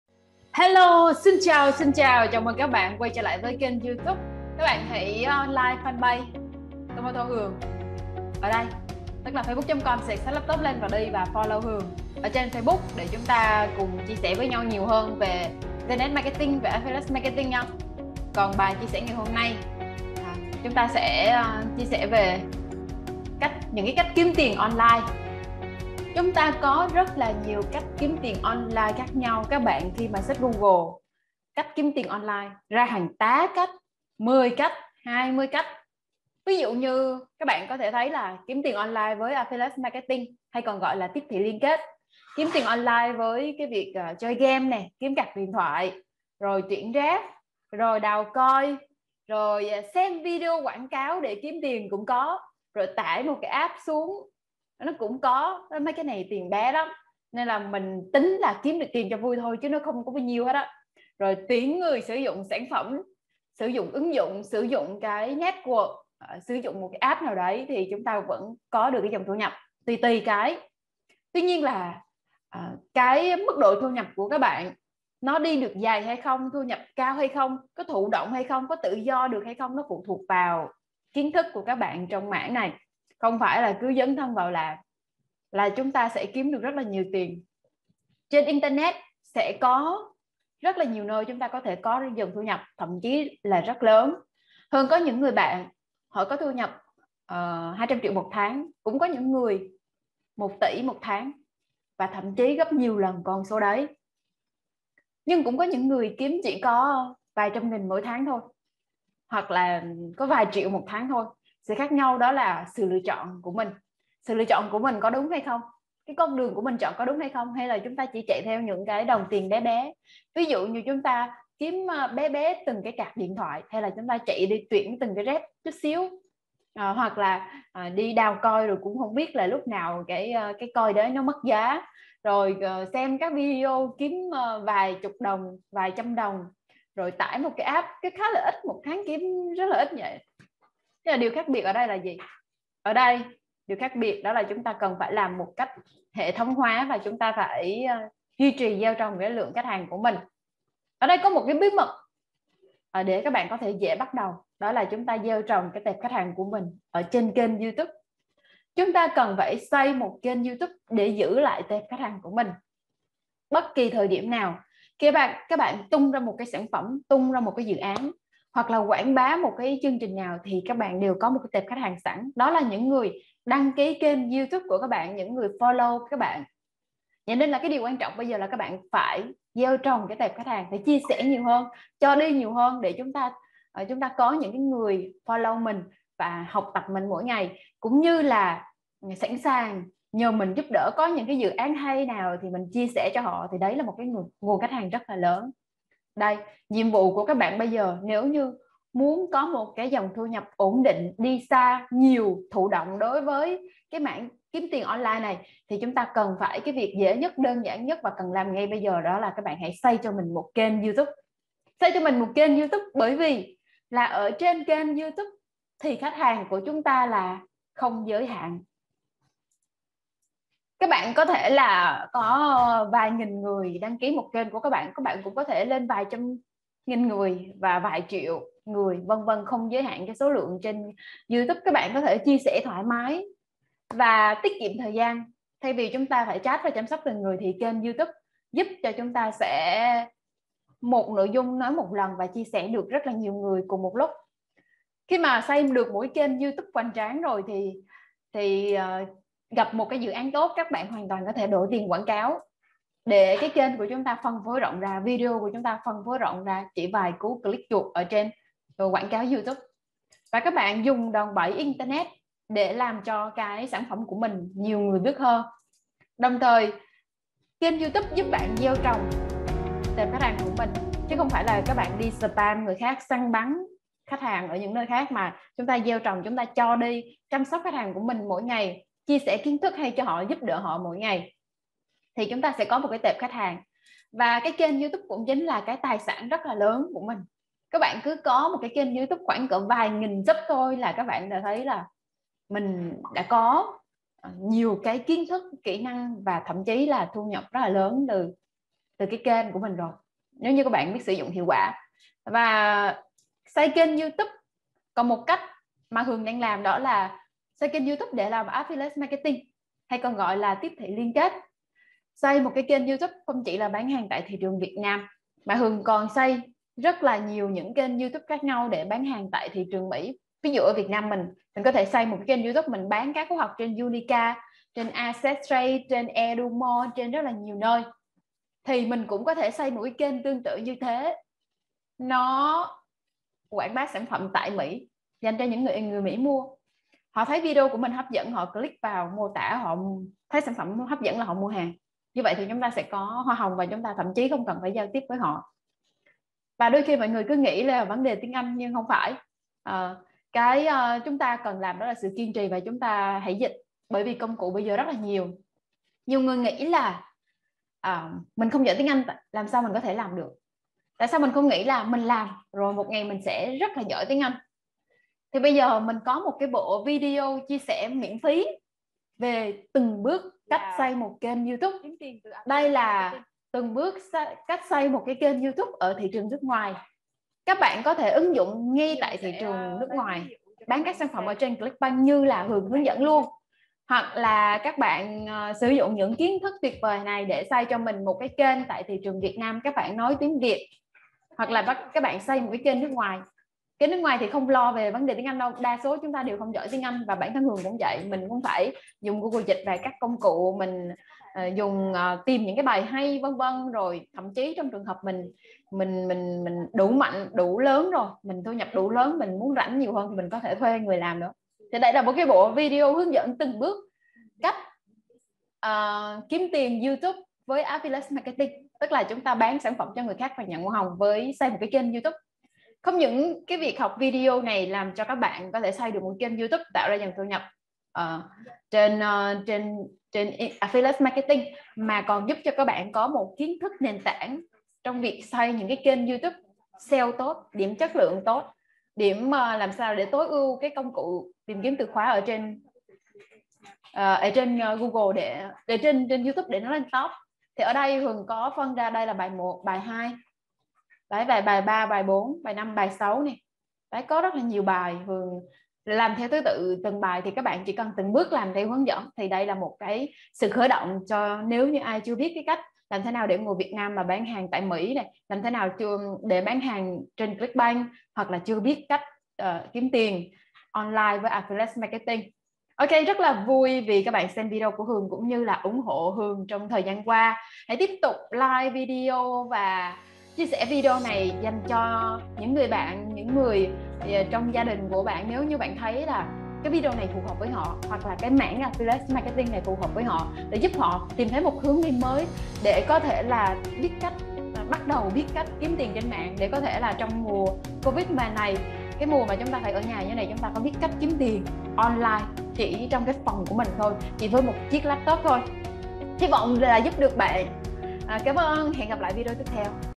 cách kiếm tiền online LÂU DÀI HIỆU QUẢ| mới 2021
Xem bộ video HƯỚNG DẪN KIẾM TIỀN YOUTUBE trên CLICKBANK tại đây
(MIỄN PHÍ)

Có rất nhiều cách kiếm tiền online, có thể: Tiếp thị liên kết, Chơi game kiếm card điện thoại, Tuyển Ref, Đào coin, Xem video quảng cáo, Tải app, Tuyển người sử dụng sản phẩm

Vì sao bạn cần xây kênh Youtube?

Khách hang không giới hạn
Dự án, sản phẩm, hệ thống tốt bạn chạy quảng cáo để dùng đòn bẫy internet
Gieo trồng tệp khách hang của mình
TÀI SẢN